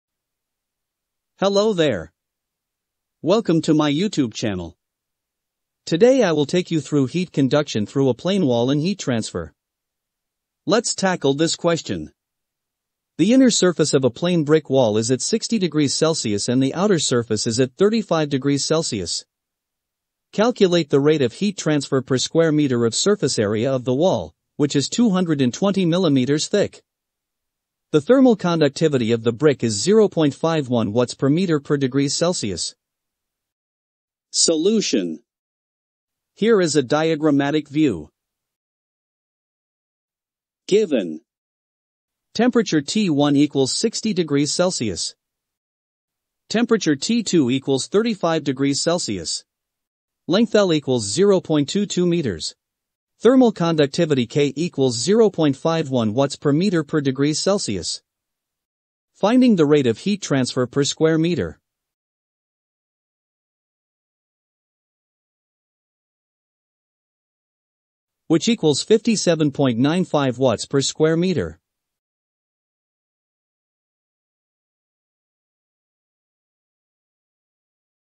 Question Solution on Heat Conduction through a Plane Wall
Are you struggling with heat conduction through a plane wall? In this video, we solve a heat transfer problem step by step, applying Fourier’s Law and thermal resistance concepts to make it easy to understand.

📌 What You'll Learn:

✔️ Basics of heat conduction
✔️ Fourier’s Law explained
✔️ Thermal resistance in a plane wall
✔️ Step-by-step numerical solution

This video is perfect for engineering students studying heat transfer, thermodynamics, or mechanical engineering.

💬 Comment your doubts, and we’ll help you out!